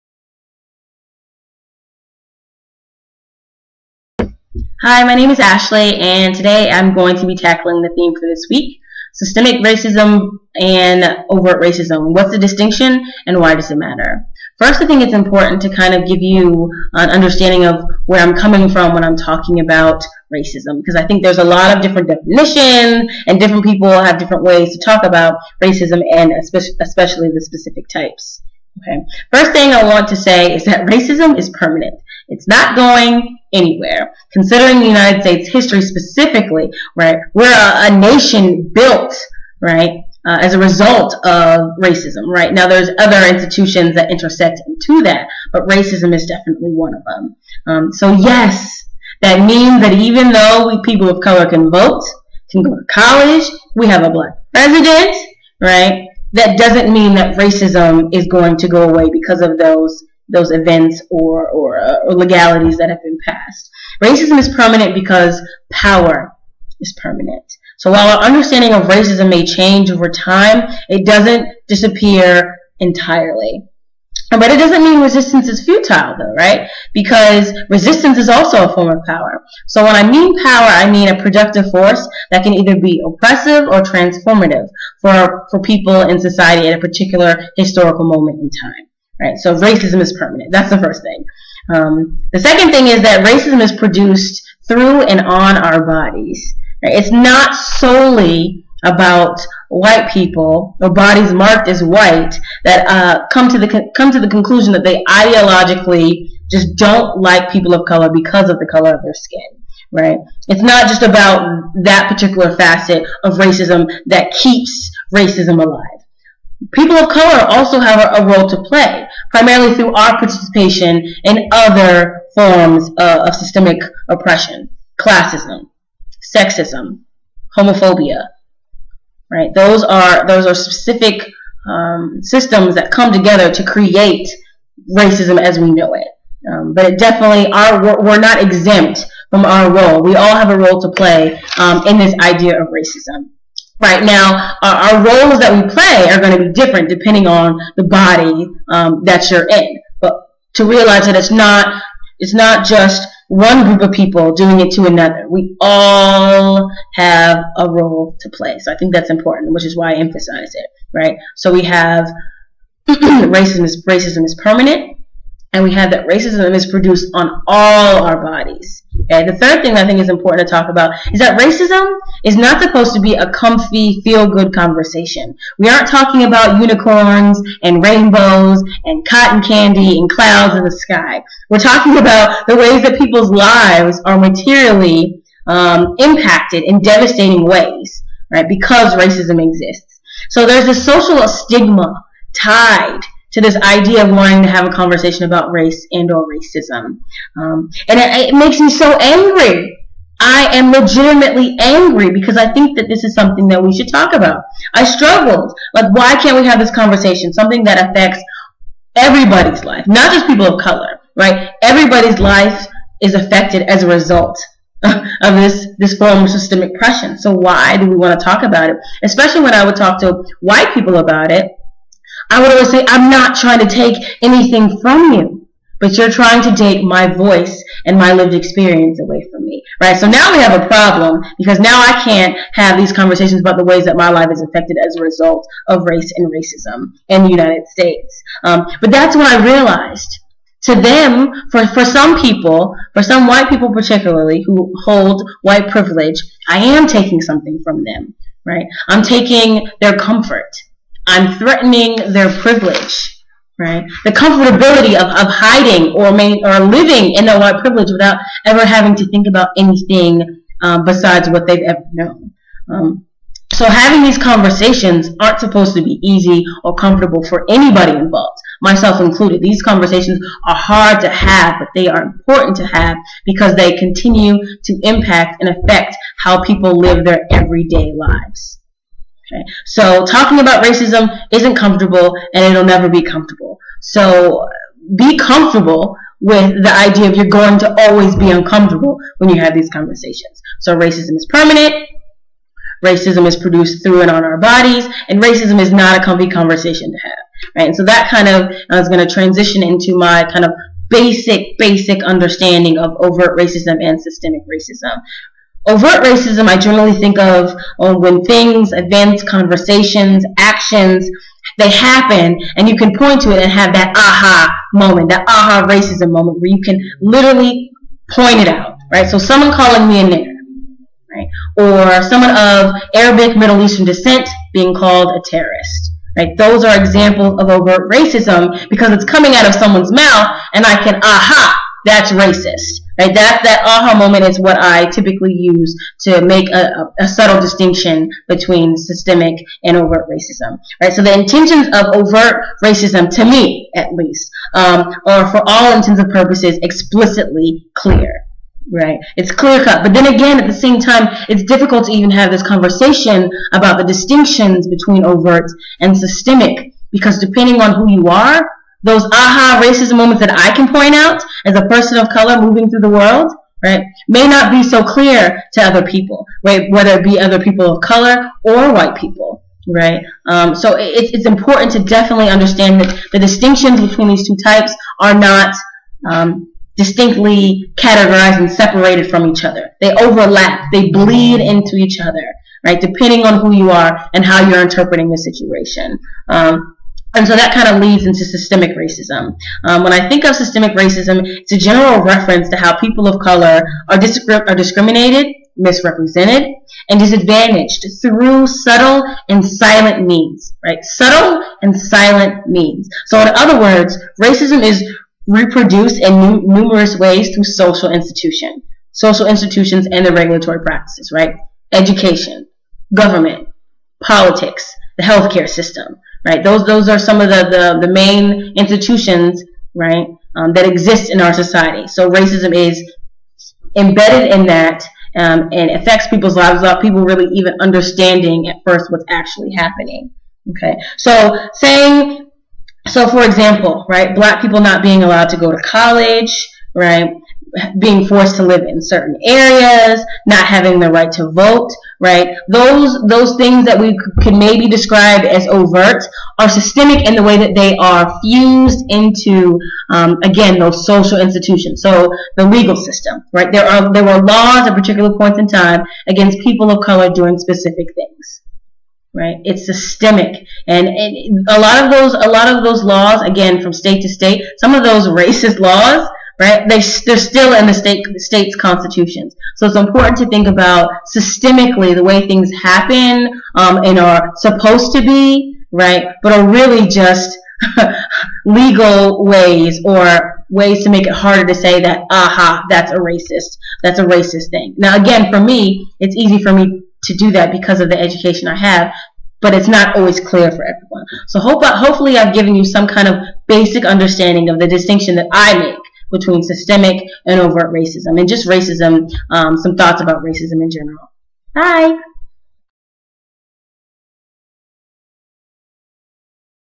Week 5: Intersections of Race and Racism
Week 5 (9/10-9/14): Topic set by Benny and LaCharles. Speak on the interplay between racism as an overt act and racism as a systemic practice that we are all engaged in. In what ways do you see racism produced in the forthcoming election, in media, and in everyday life. What does it look like to intervene in these discourses.